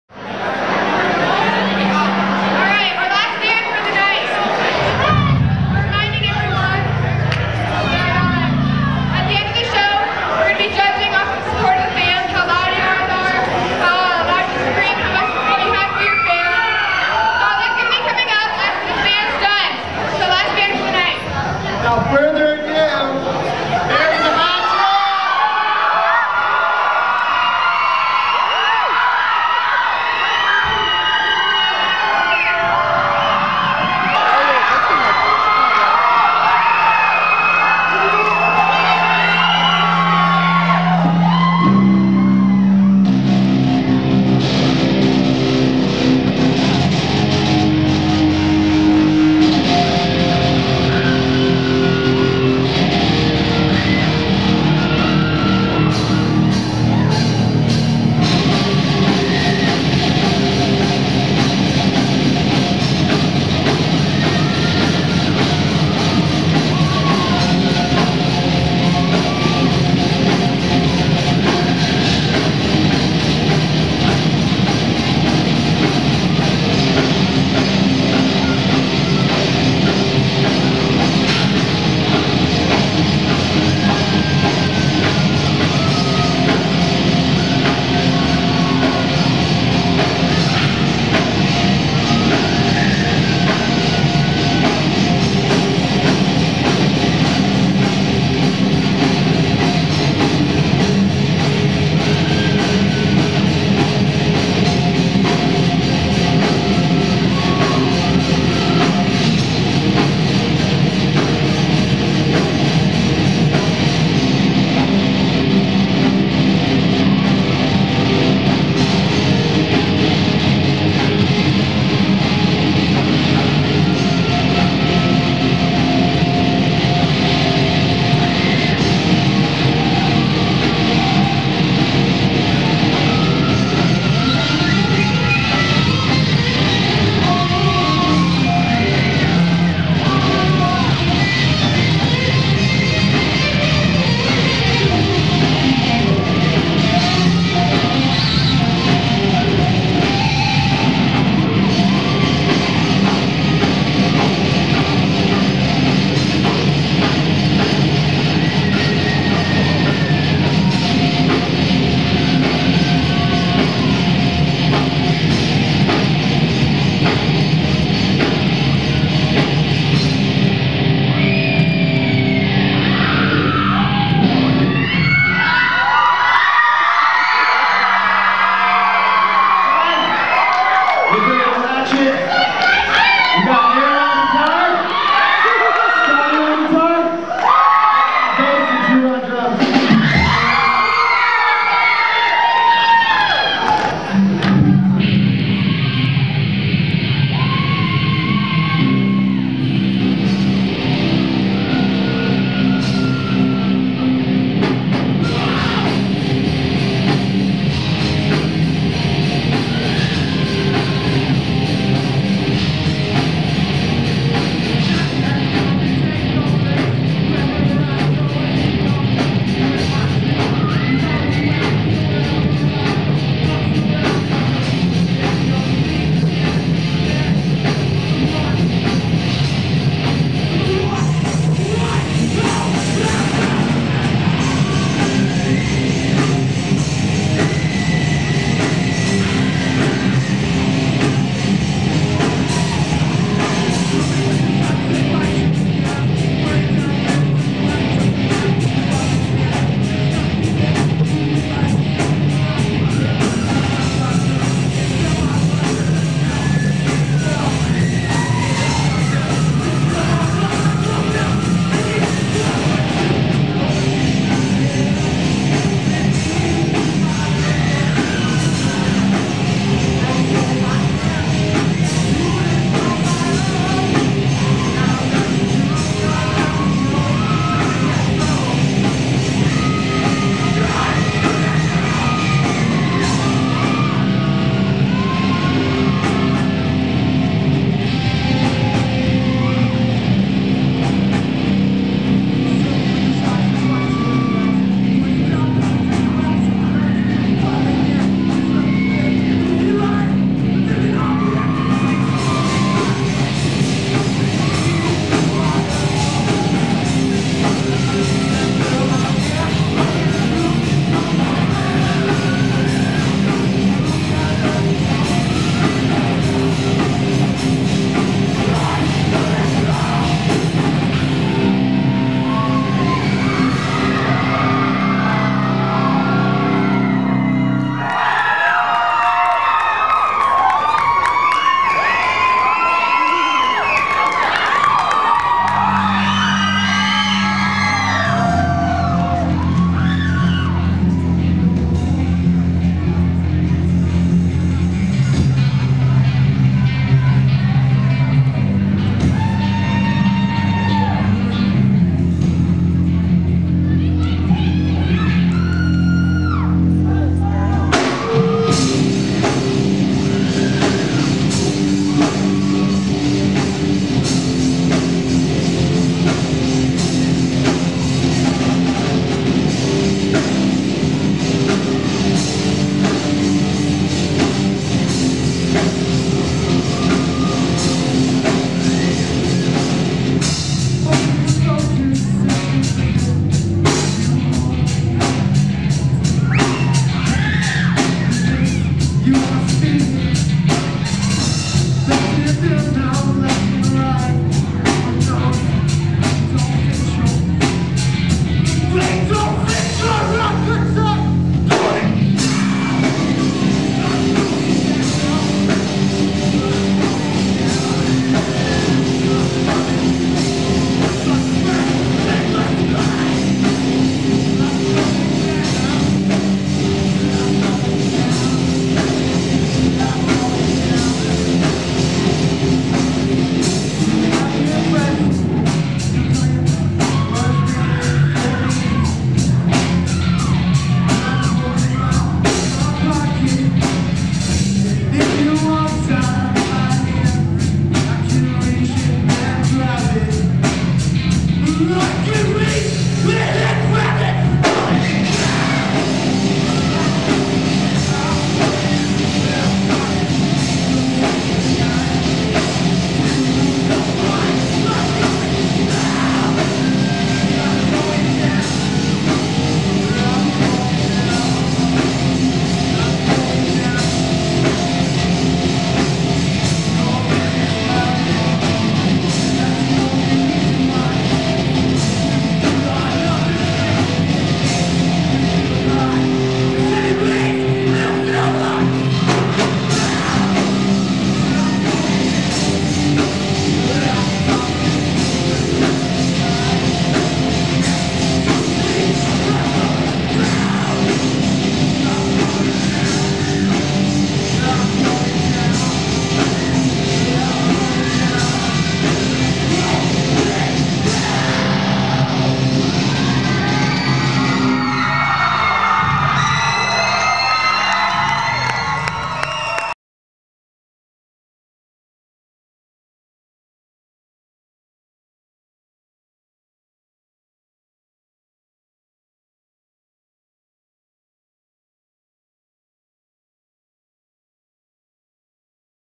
Bury The Hatchet™ - Part 1
Battle of the Bands in Longmont, Colorado on January 11, 2007.  Bury the Hatchet™ took first place.